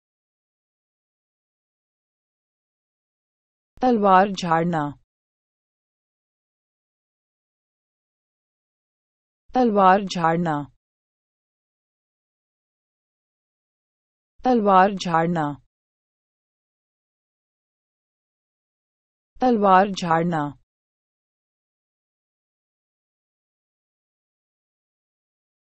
Talavaar Jhaarana pronunciation in Urdu | Pronounce Talwar Jharna in Hindi | تلوار جھاڑنا - تلفظ
Talavaar Jhaarana, Talwar Jharna pronunciation in Urdu/Hindi
How to pronounce Talavaar Jhaarana in Hindi/Urdu
تلوار جھاڑنا کا اردو/ہندی تلفظ